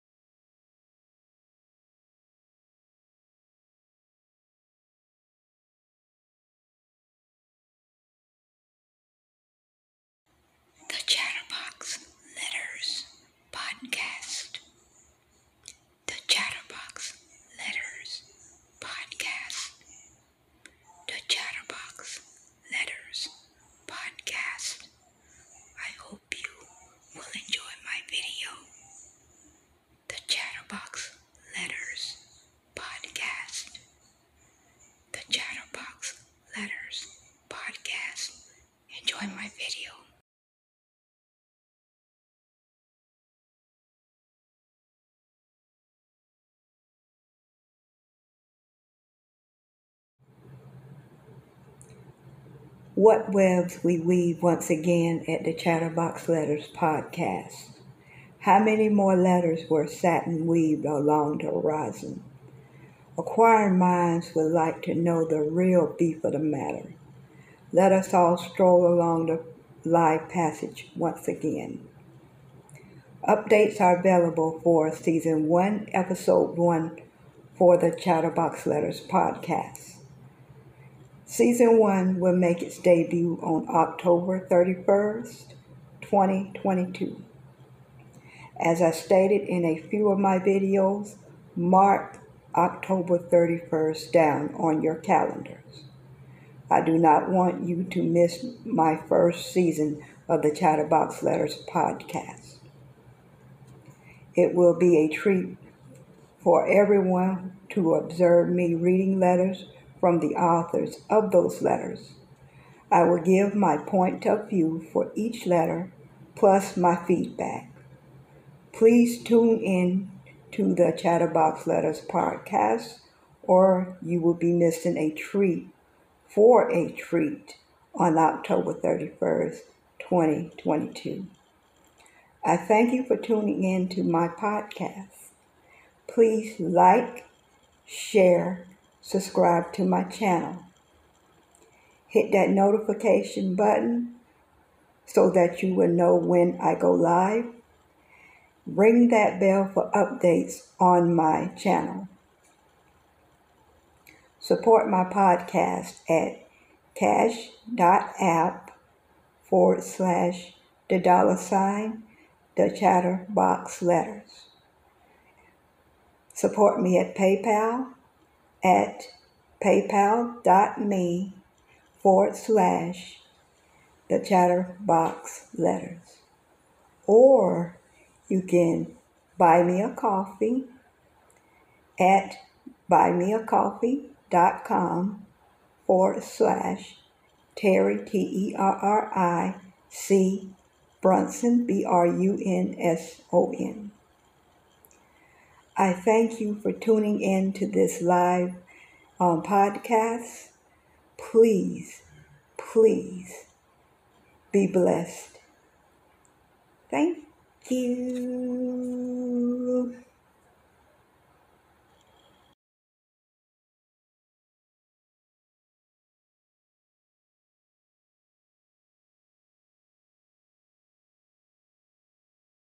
The Chatter Box Letters Podcast 🎙️ Season 1 Of The Chatter Box Letters Podcast 🎙️
On Season 1 Episode 1 Will Debut On October 31, 2022. 🎙️

The Chatter Box Letters Podcast Is An Open Letter Business Where Clients Send Their Letters In To Be Read Through Public Access. 🎙️ Letters Will Consist Of Any Problems, Issues, Or Anything Good Or Bad That Need A Feedback Response From The Chatter Box Letters Host Author Terri Celestine Brunson. 🎙️ NOTICE: No Personal Aspects Should Go Into The Letter! 🎙️ NOTICE: Not Trying To Be An Extreme Problem Solver, Just Like To Help With The Problems, Issues Good Or Bad You May Have 🎙️